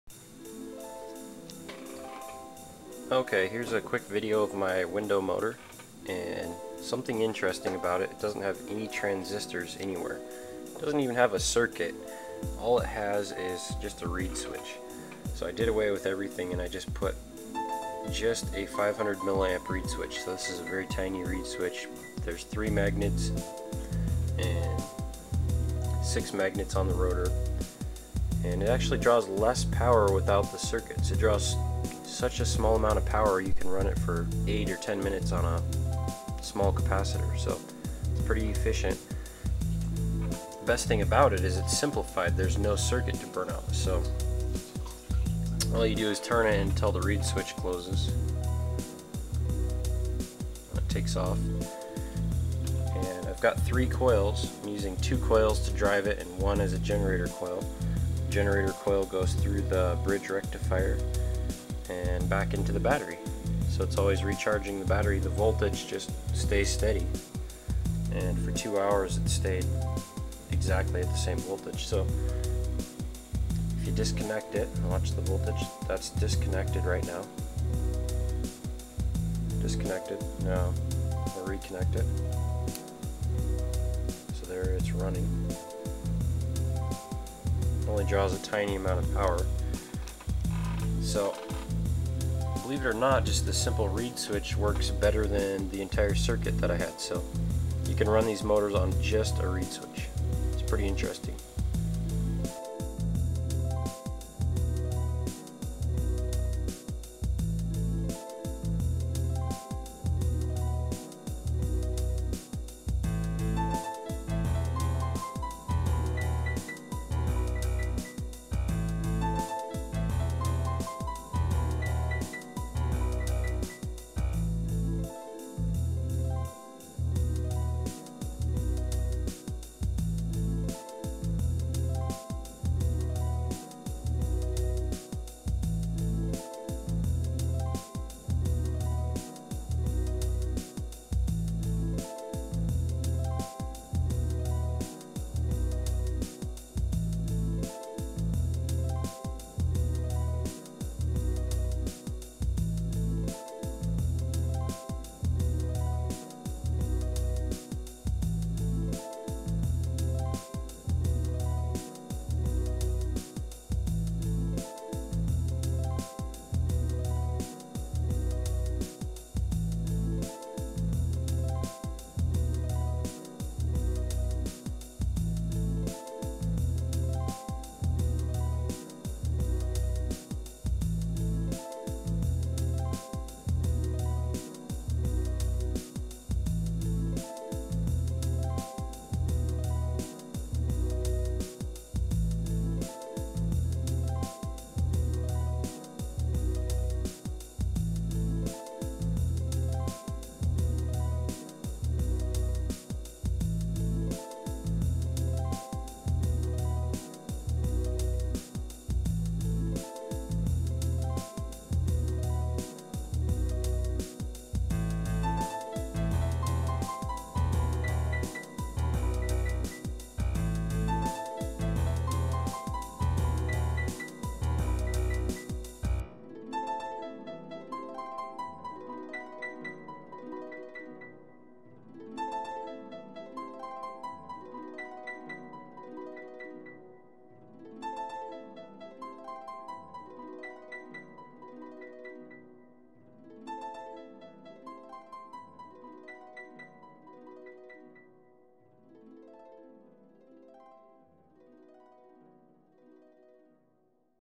Bedini-Cole window motor - Mechanical Switching
No transistors. No circuit. Just 1 reed switch and one rectifier.

Please take a look at the Ritalie.com "Free Energy Webstore" for more information on projects like this.